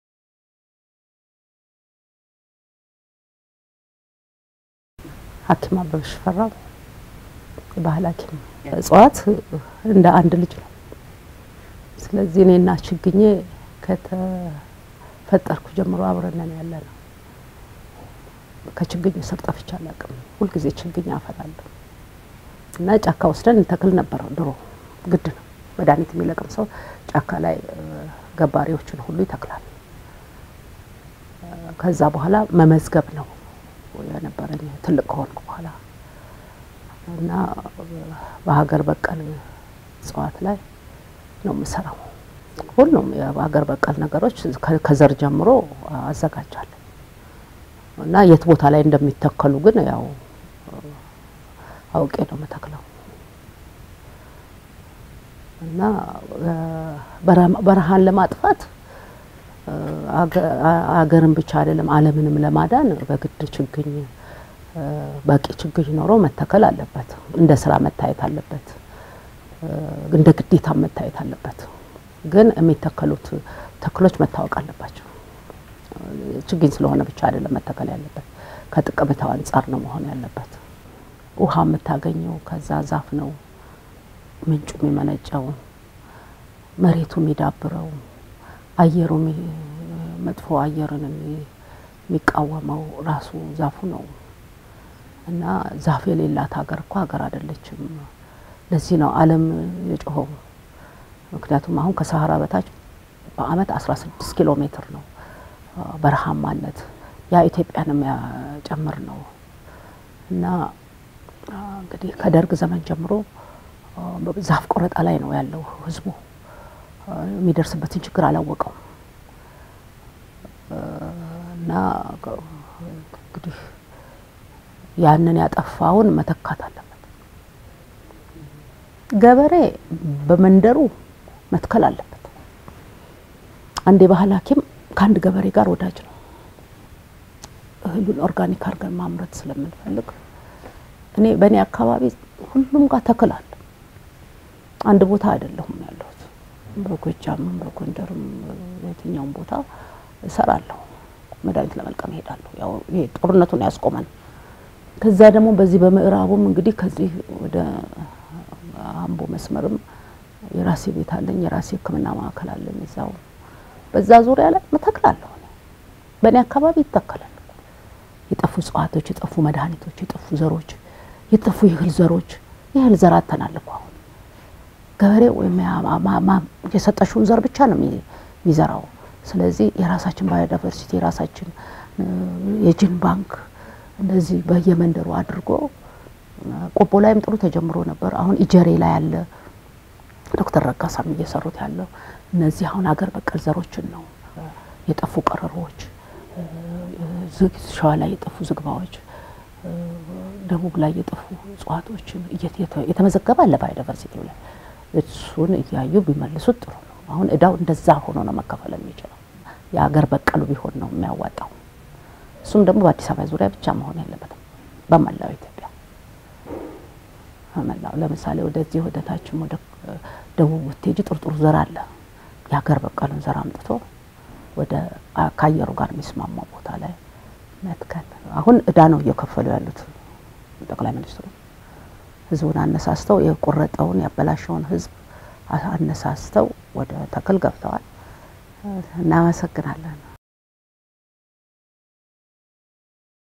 የዕፅዋት ጥበብ!!
በረሀማነትን በመከላከል ጤናማ  የአየር ንብረት እንዲኖር  ህብረተሰቡ  በየአካባቢው እጽዋትን መትከል ባህሉ  ሊያደርግ ይገባል፡፡